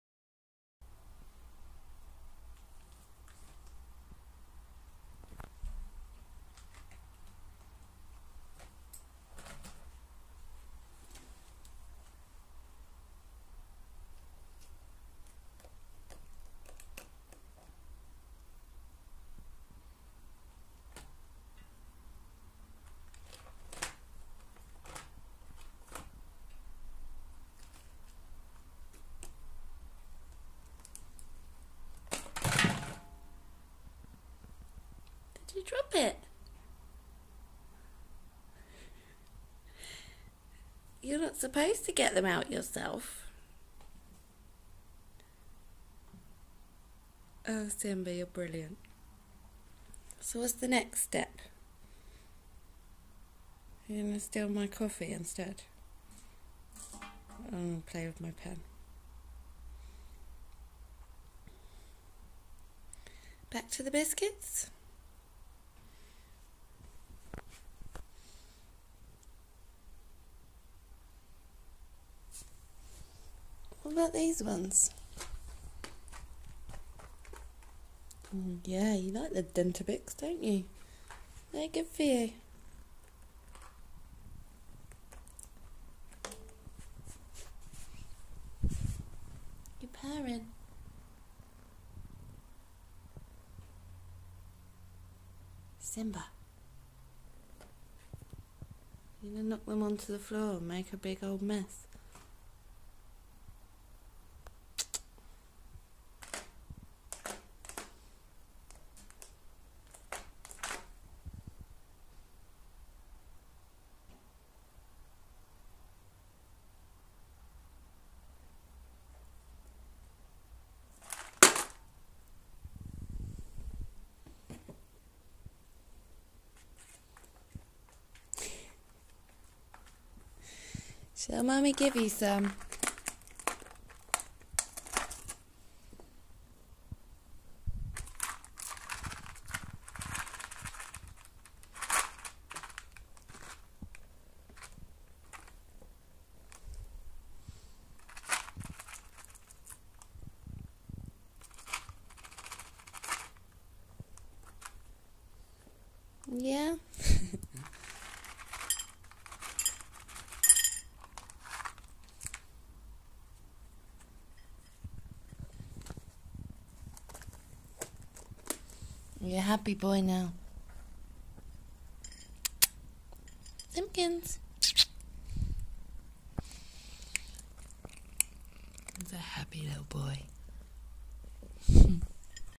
Whiskas Dentabits & Temptations Quest feat. Simba
My little simba loves his Whiskas treats and will even try to get them for himself :-)